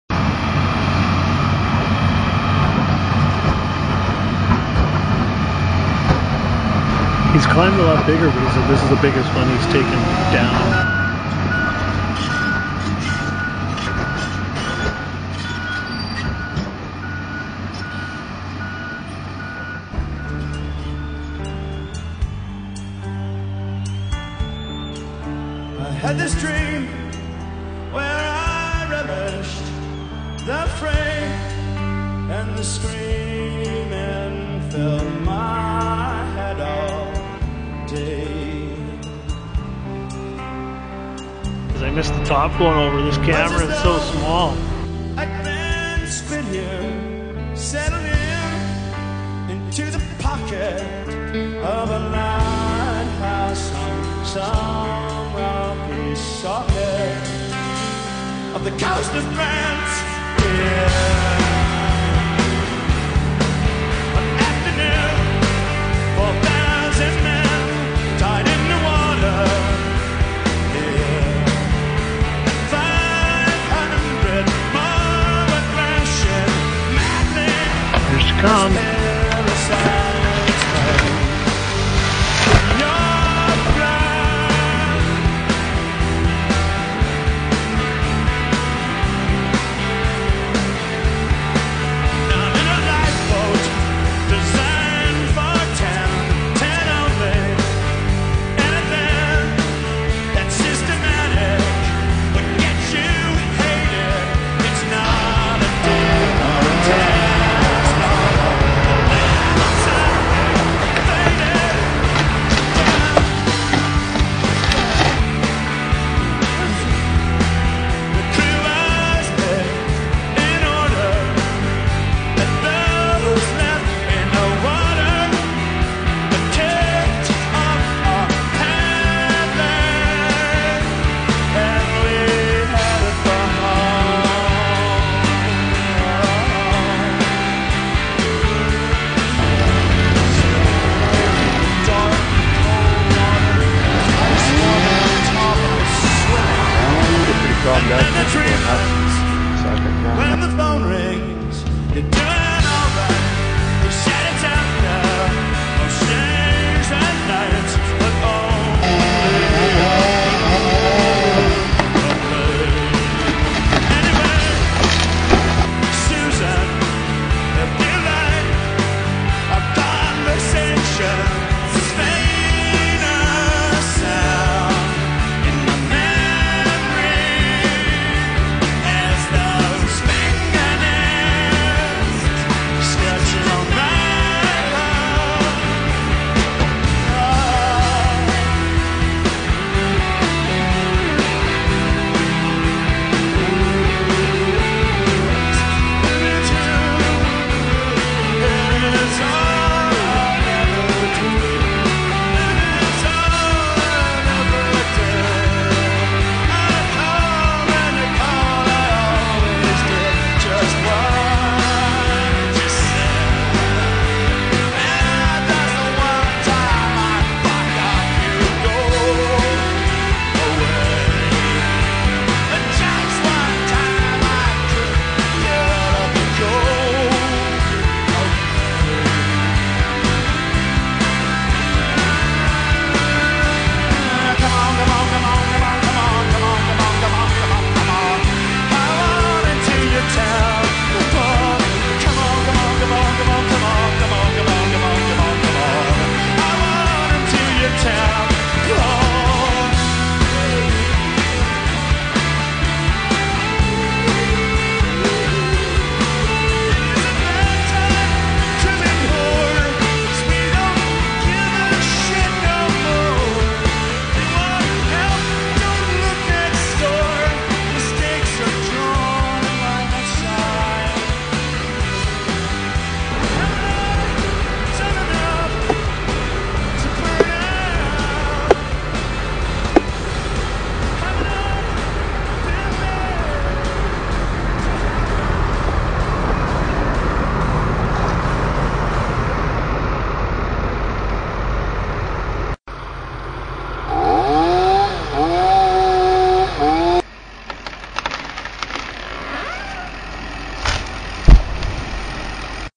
Old Growth Fir Take-Down
Piecing down an old Douglas-fir. Unfortunately a road for a housing development is being built too close to this tree to let it stay. Hundreds of years of growth ending in a few hours.
About 65" DBH and I would estimate just over 180' tall.